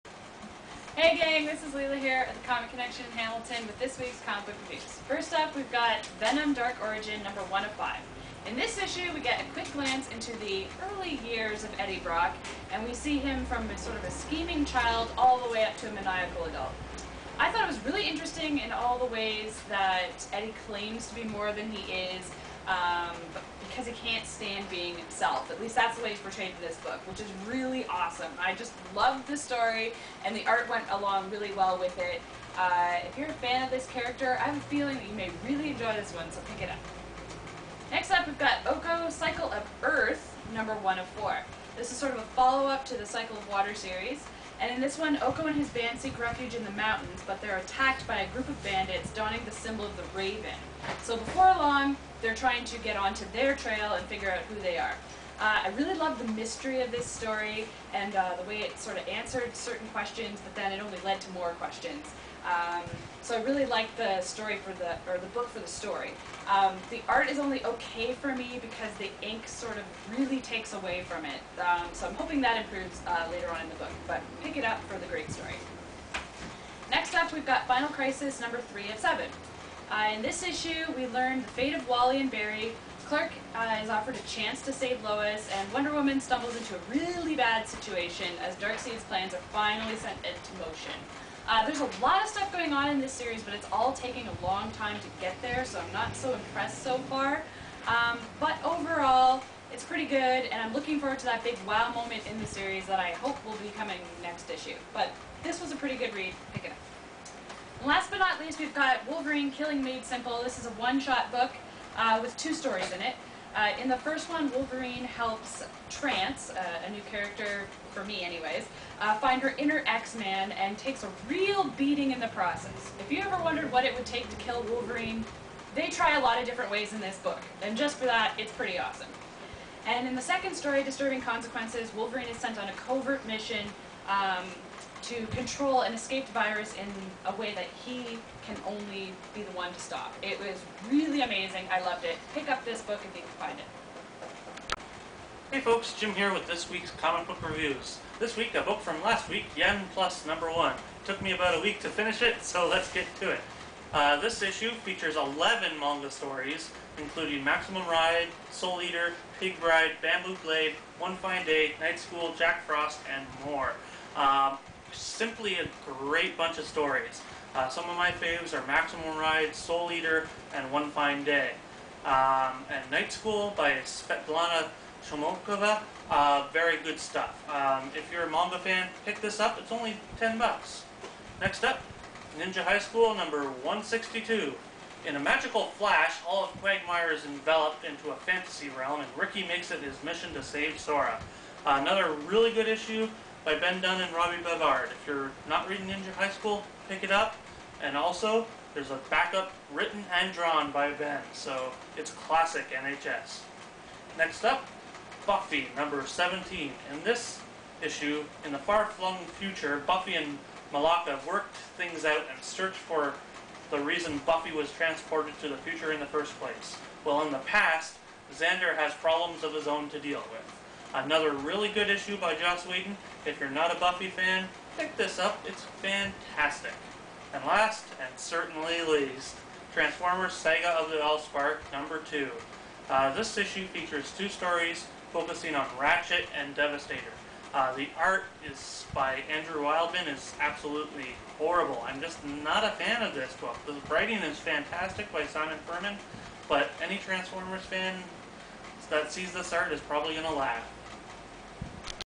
Geeking Out Comic Peek O' The Week 08/06/2008
On this weeks episode Jim & Leila review eight new titles including: Yen Plus # 1, Venom Dark origin # 1, Ninja High School # 162, Okko # 1, Buffy The Vampire Slayer # 17, Final Crisis # 3, Transformers : Saga of the all spark # 2 and Wolverine : Killing made Simple # 1.